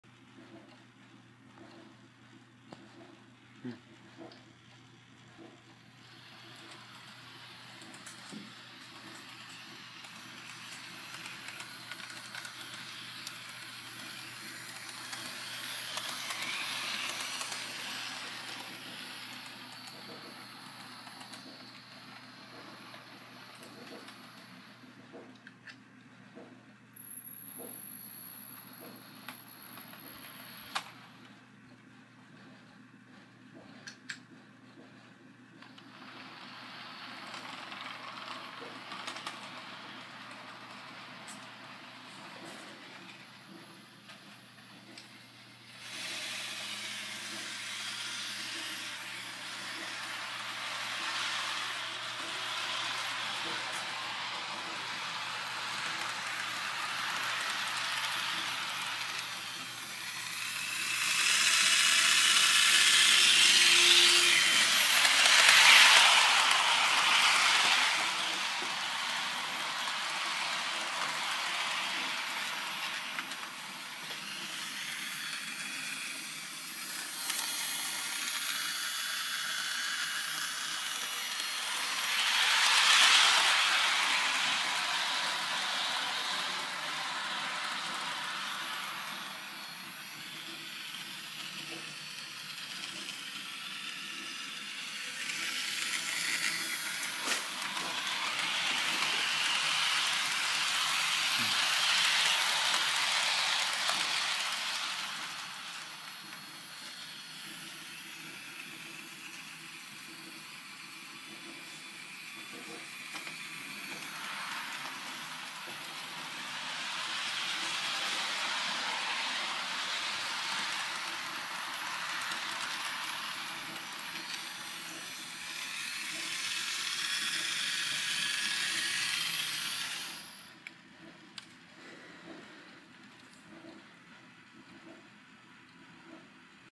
Bachmann Koko
via YouTube Capture the 2nd video shot with my iPod. So here is a test with my new Bachmann Koko from chuggington that I got for my birthday. I've altered her slightly as usual the only chuggington models I'm gonna get are Zephie, Piper and Decka. Anyways Enjoy!